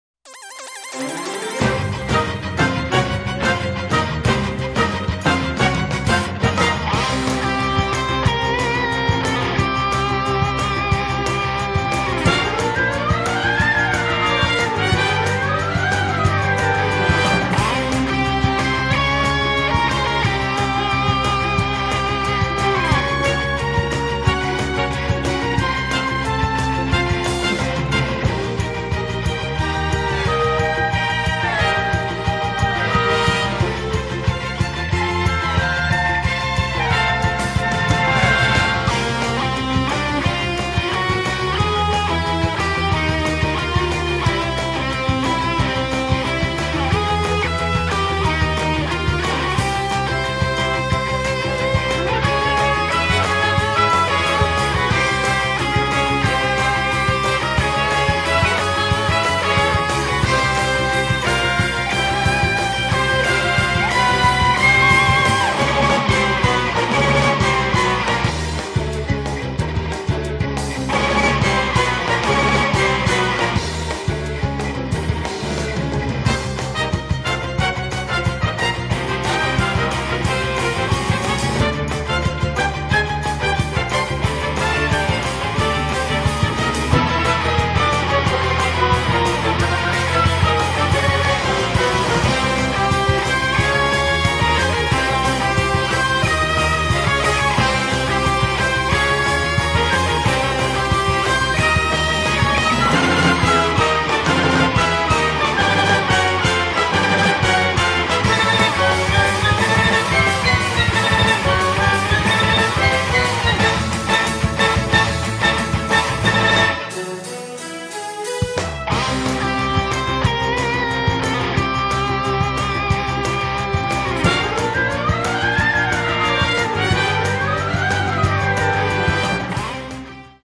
[Unboxing] Jigglypuff & Greninja amiibo
The Pokemon amiibo are here! We unbox both Jigglypuff and Greninja in this video.